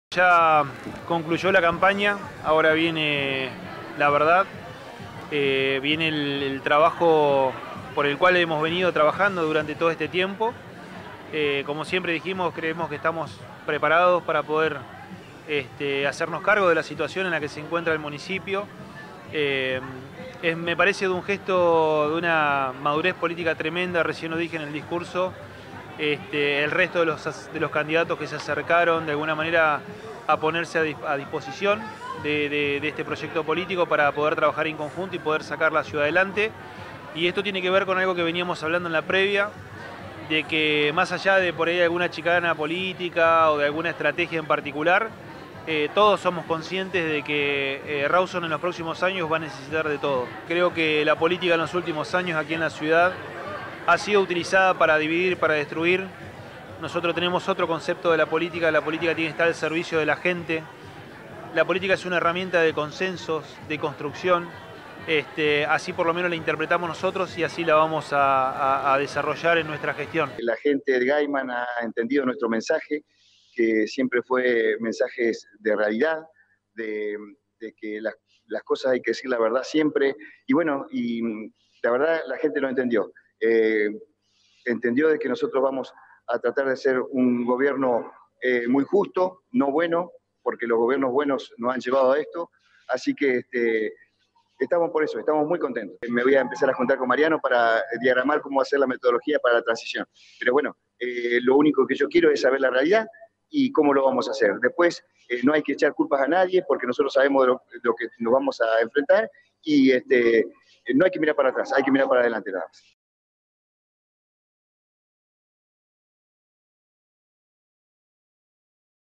Tema del Día en 12 Noticias
Chubut eligió
Este domingo entrevistamos al intendente electo de Rawson, Damián Biss, luego de dar sus primeras palabras confirmando su victoria.
También hablamos con Darío James, intendente electo de Gaiman, que se encontraba feliz porque "la gente comprendió su mensaje"